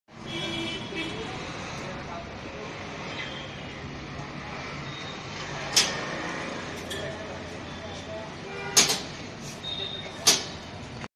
Full Height Turnstile With Finger Print Reader
Full Height Turnstile With Finger Print Reader installed by KANHAGROUPS